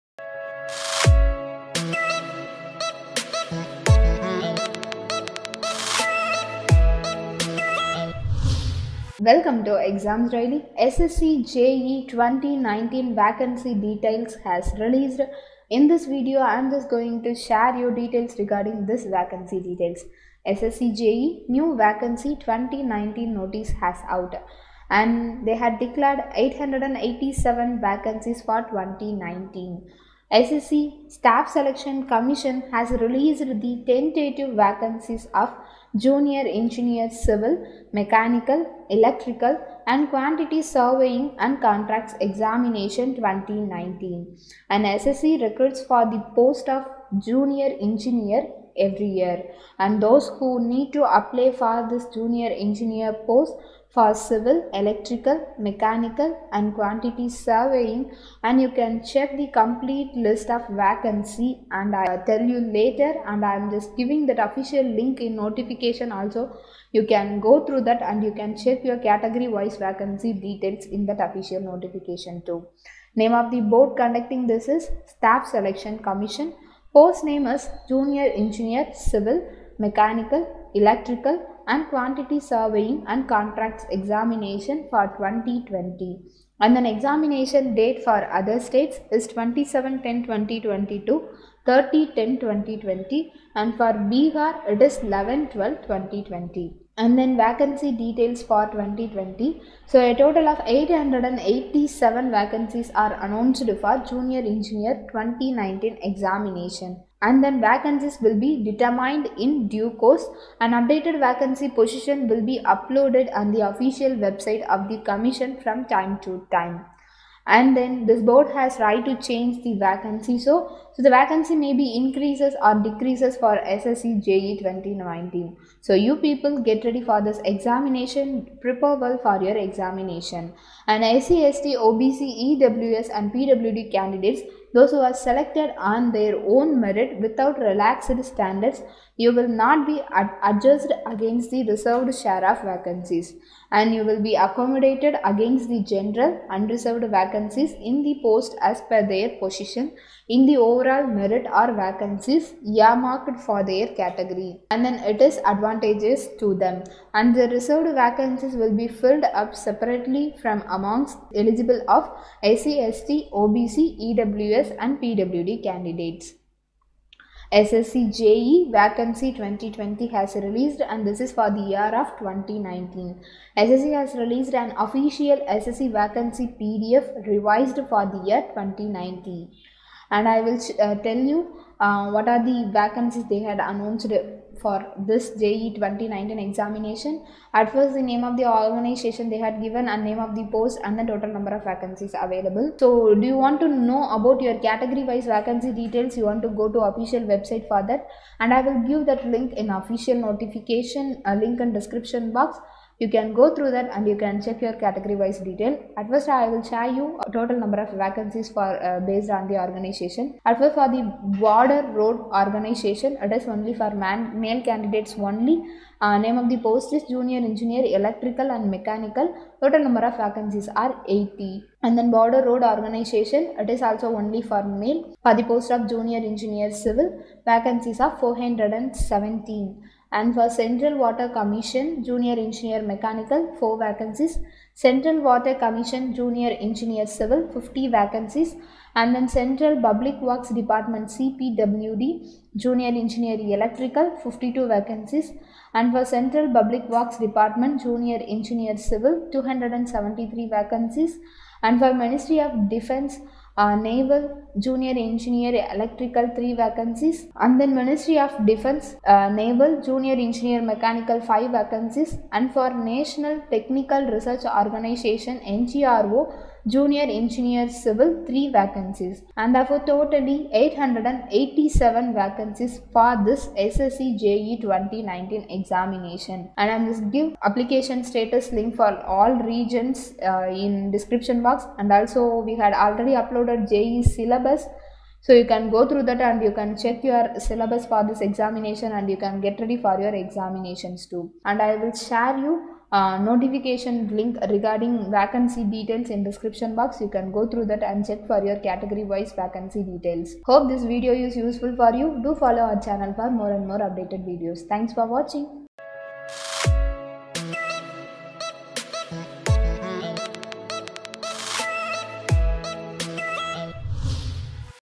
SSC JE 2019-20 Vacancy | SSC JE Tentative Vacancies | Mechanical, Electrical, Civil Details
Staff selection commission has released the tentative vacancies details in the official website. ssc je (junior engineer) mechanical,electrical,civil department wise tentative vacancy details in this video. let subscribe our examsdaily channel and stay active with us for more updates.